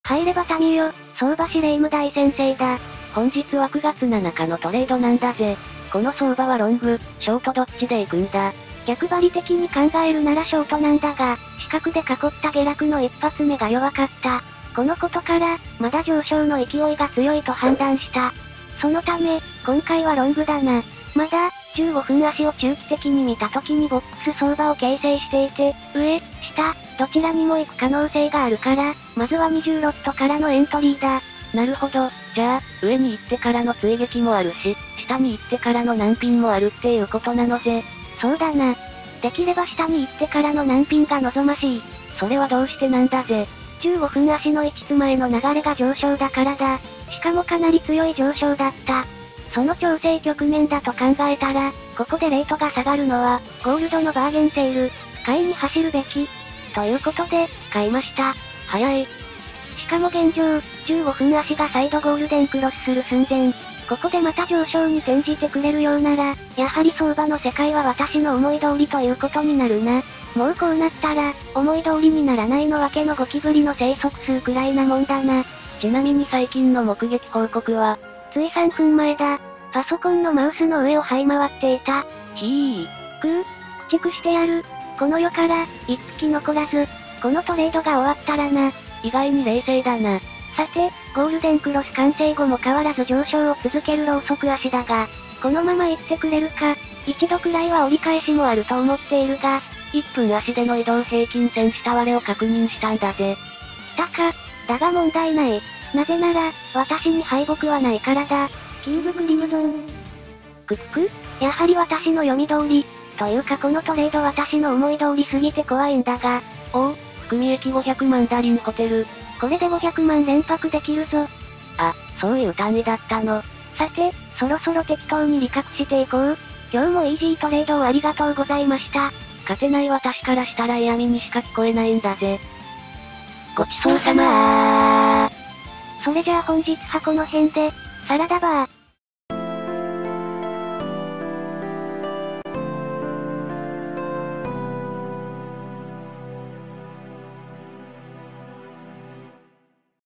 【実演教材】国宝級ナンピントレードで500万円爆益利確！！ゴールドFXなら知っておきたいトレード手法
公式ラインで限定情報をゲット！（EA、裁量ほしい人は必ず登録して下さい！）

霊夢が使っているFX業者さんはFXBeyond！
下記URLより口座開設で入金ボーナス１００％！！